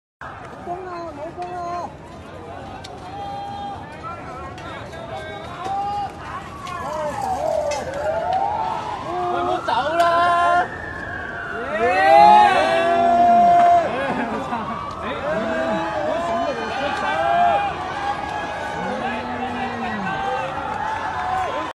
【理大現場編委報導】
[18:52] 有數架警車駛入理大A Core，示威者表示歡迎，但未幾警車掉頭駛走，示威者喝倒采。